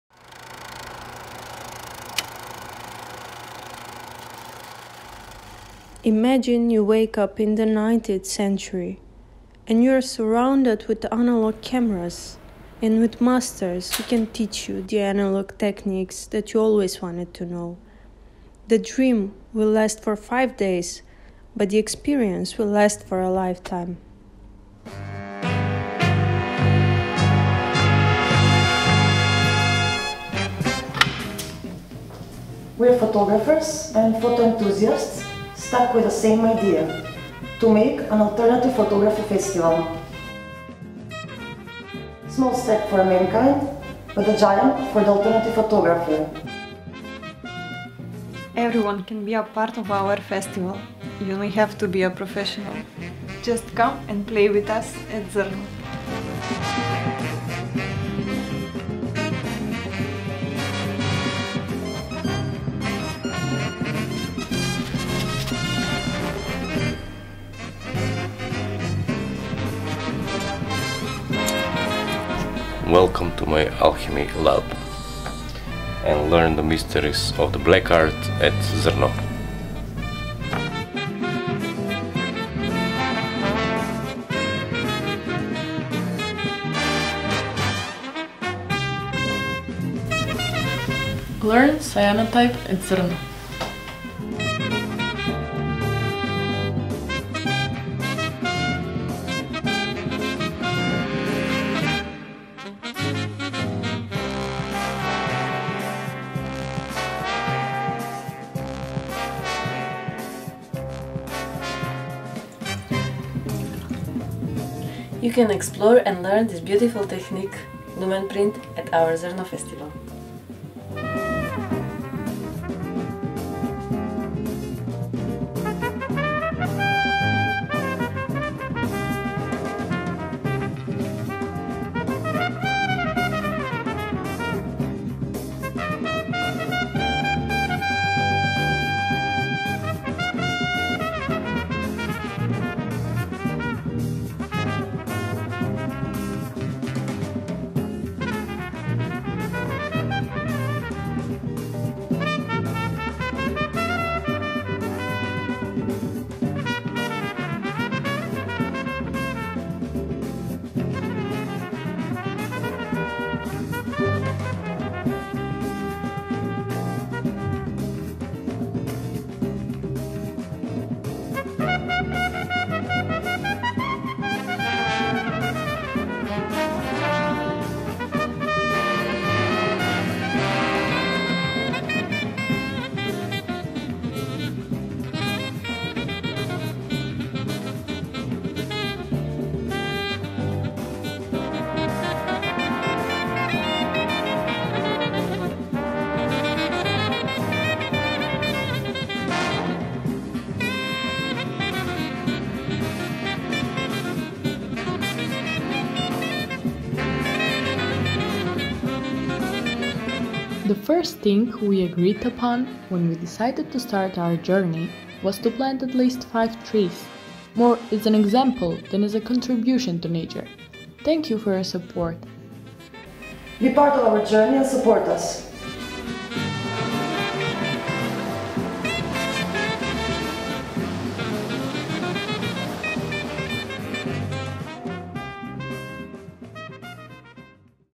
SUPPORT OUR ADVENTURE of PHOTO PLEASURES
promo video about first edition of ZRNO - international festival of alternative photography